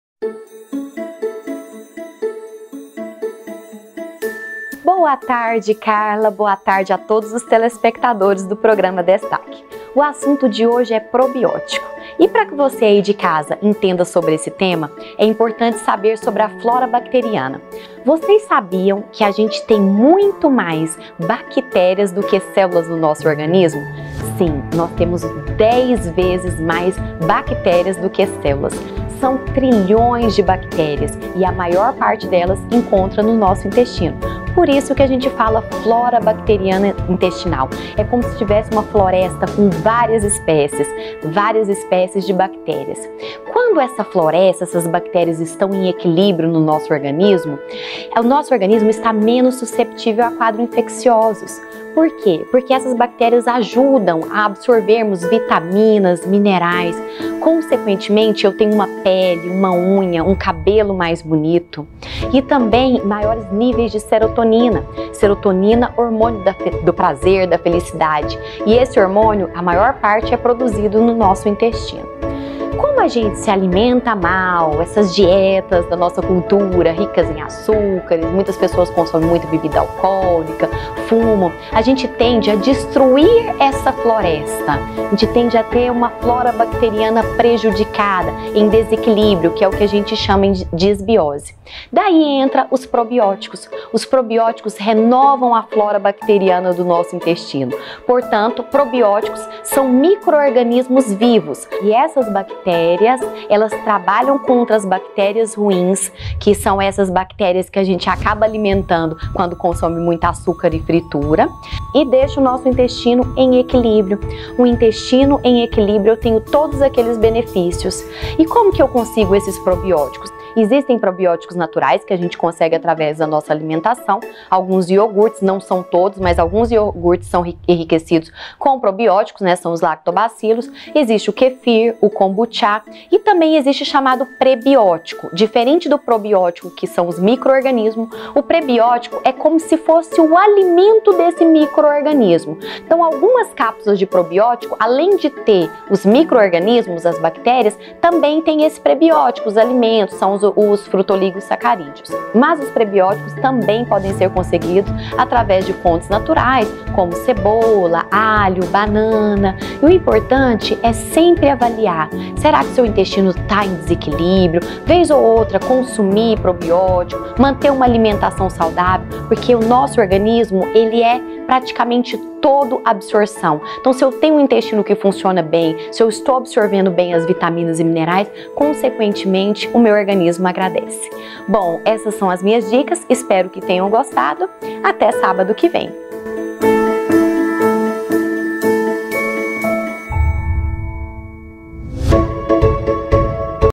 DESTAK COM CARLA GOMES | PROBIÓTICOS (DRª THAÍS AQUINO)
Você sabe o que são PROBIÓTICOS? A Drª Thaís Aquino explicou direitinho o que são e quais são os seus benefícios para uma boa saúde.